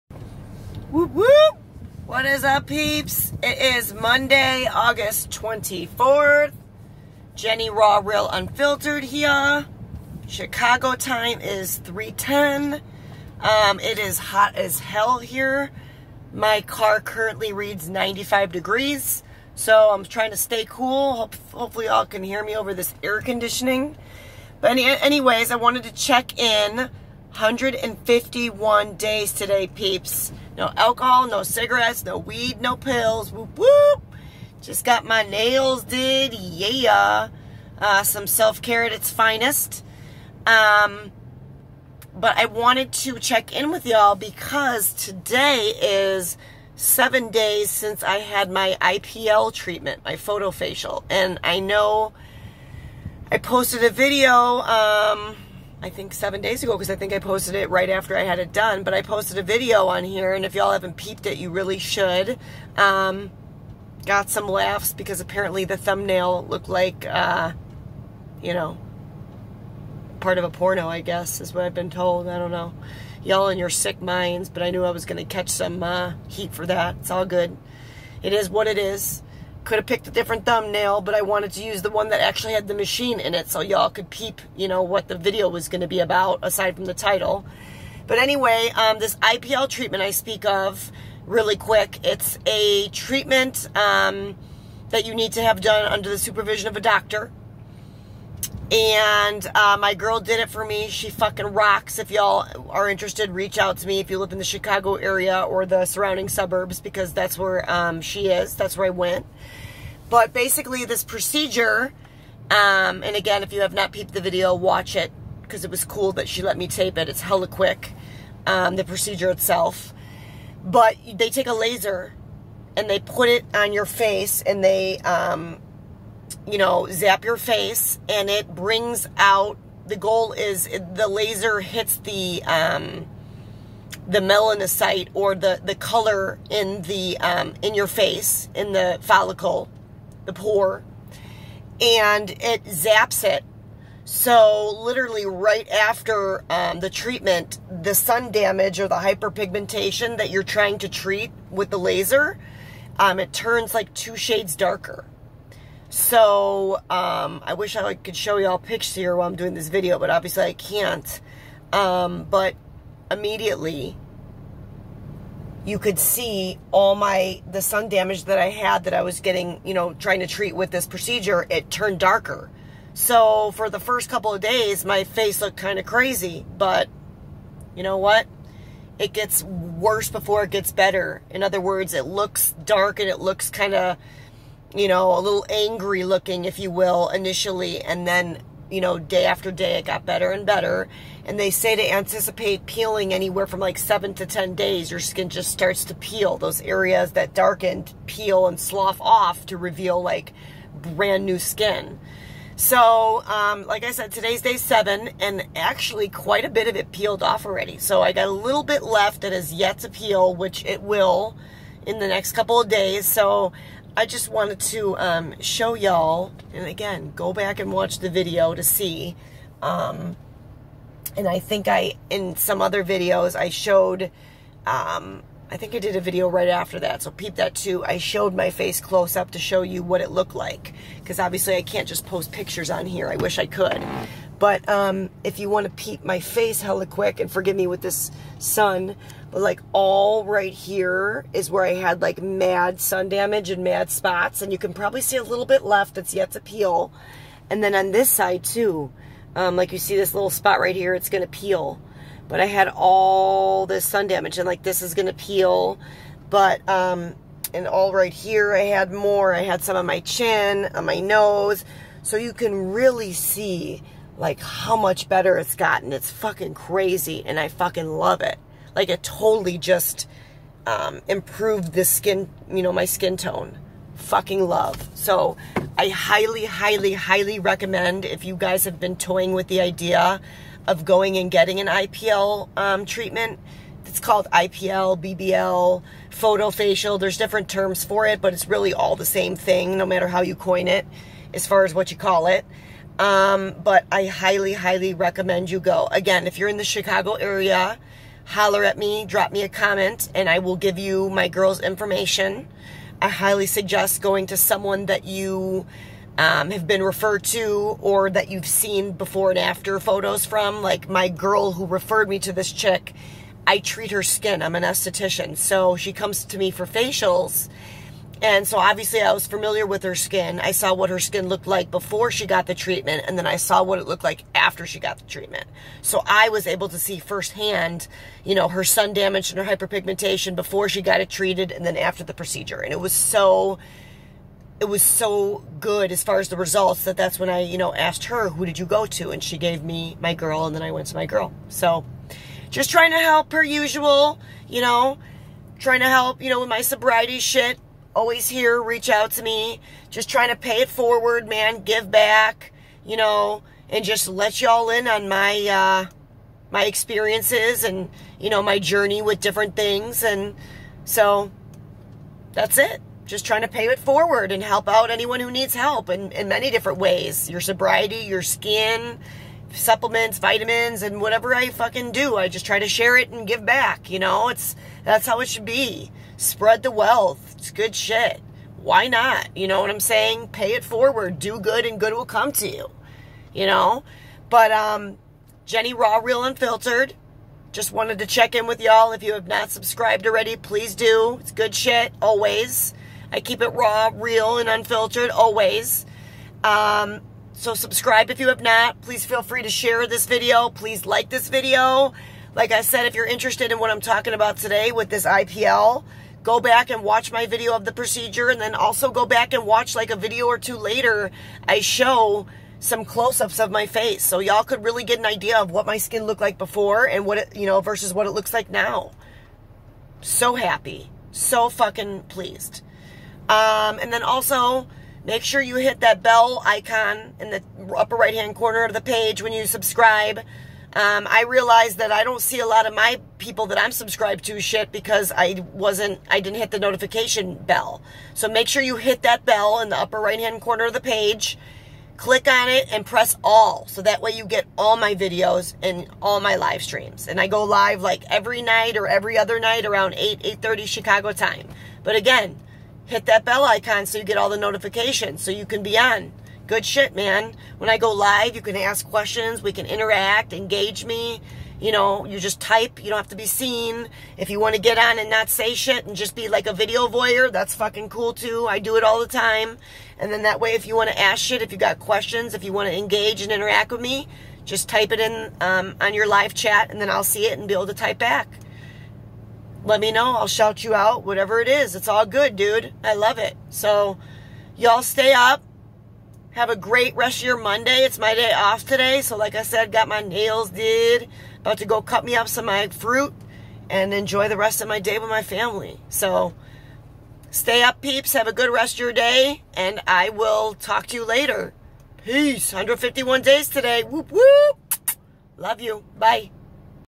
Look at my face!! 7 days post IPL procedure/photo facial!!
A week ago I dropped a video of me getting a photo facial to get rid of my sun damage/hyperpigmentation!! Peep the results so far. I'm sooo PUMPED!